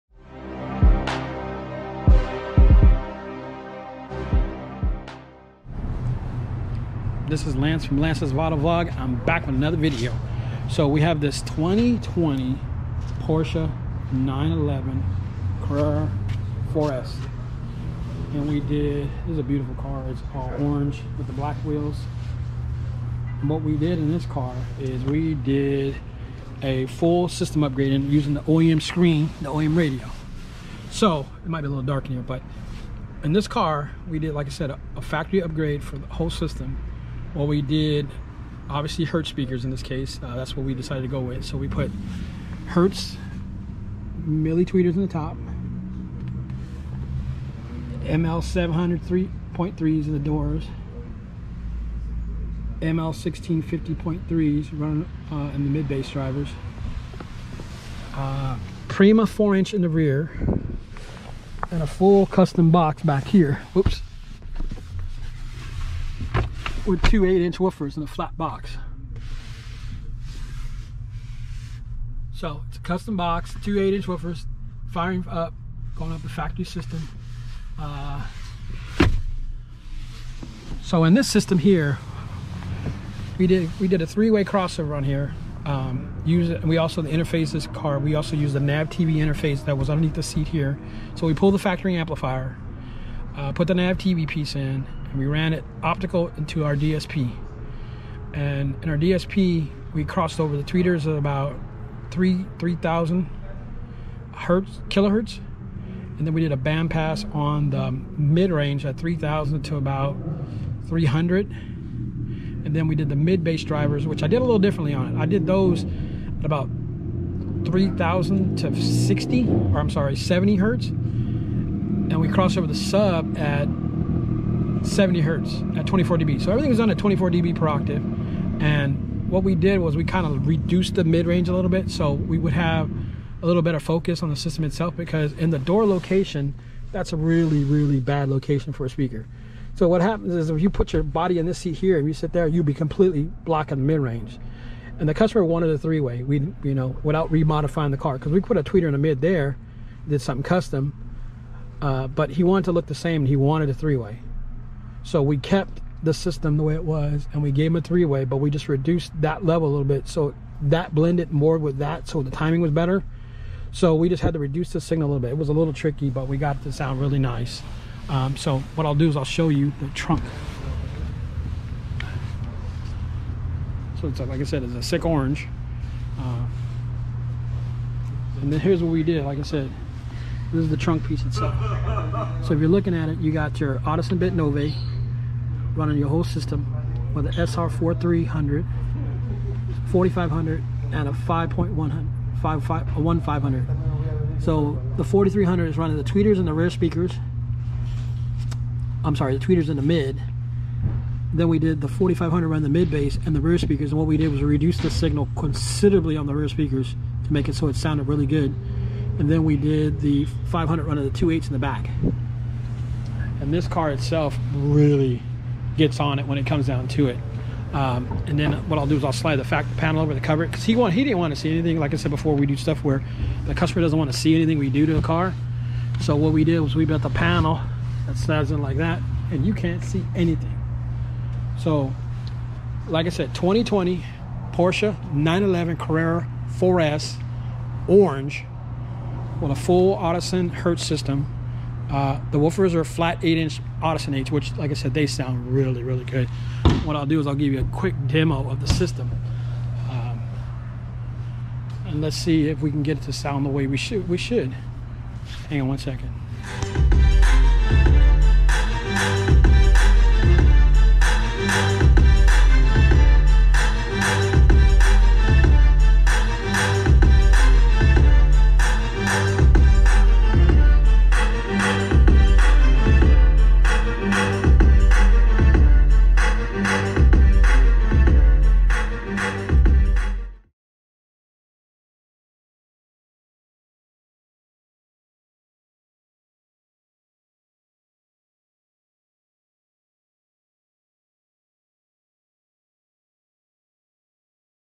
Audison & Hertz car audio does make this 2020 Porsche sound awesome!
In the upgrade didn't replace the radio. Radio factory radio had all the features that the customer wanted.
So we need a interface to integrate the source unit to what we wanted to make work. The answer was a NavTv Zen module. This interface had a optical output in which  allows us to have the best possible output.

Now that interface module is determined, we can move on to the DSP. The DSP we choose was a Audison Bit Nove. This unit has 9 channels of output and that's what we needed to run the system fully active.

With the DSP decided it was time for amps. Audison is our go to amps. We needed something compact and the SR series fit the bill. So the SR4.300, SR4.500 and the SR1.500 it was.

Now time for speakers. Hertz ML28.3, ML700.3, ML1650.3 legends for the front. Audison Prima APX4 for the rear & 2 APS8R for the subwoofers.

The system was installed stealth and it sounds incredible.